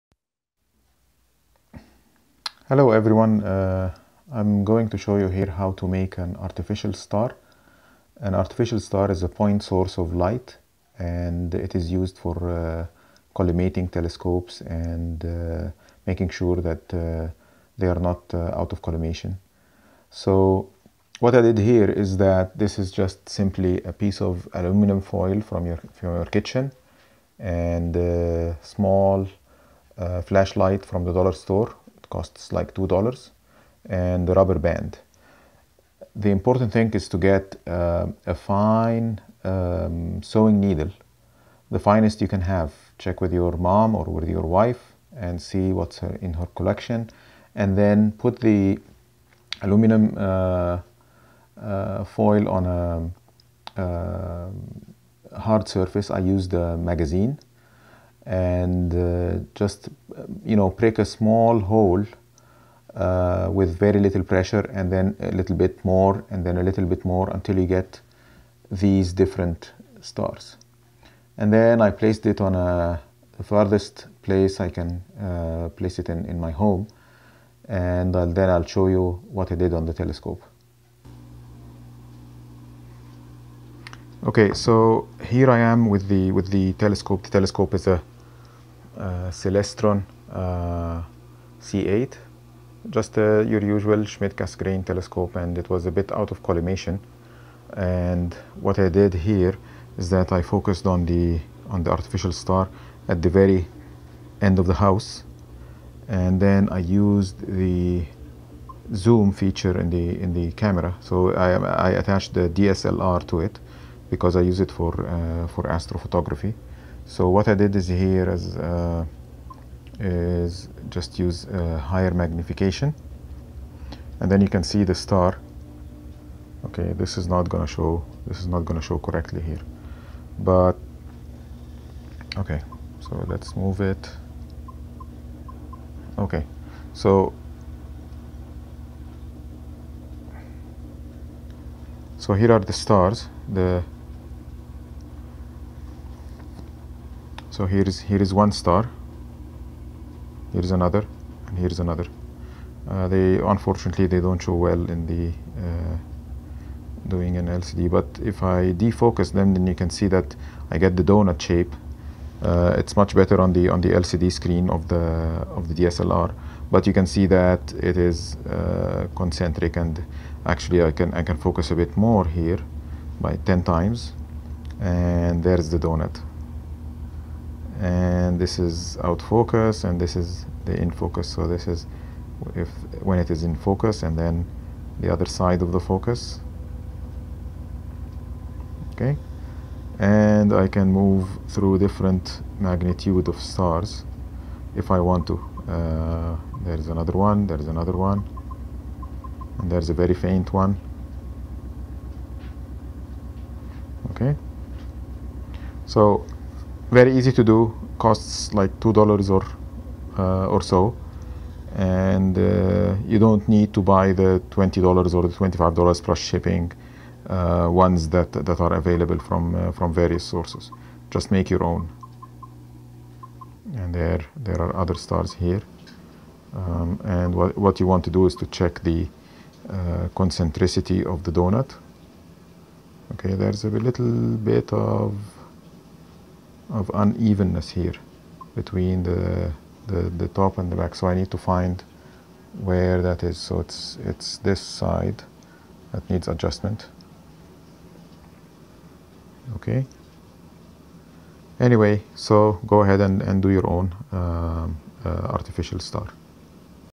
Make Your Own $2 Artificial Star For Telescope Collimation
An Artificial Star is a point source of light. It is used to simulate a real star indoors, so you can do the collimation indoors, and not waste valuable observation time.

Commercial artificial stars cost $20 or more, and their main feature is a small plate that has precision holes of 50 micron up to 250 microns.

You can make your own artificial star, which is very close in function to the commercial ones.

You need a small flashlight, which costs $2 at the Dollar Store, a piece of kitchen aluminum foil, a fine sewing needle and a rubber band.

The full instructions on how to make your own can be found on my web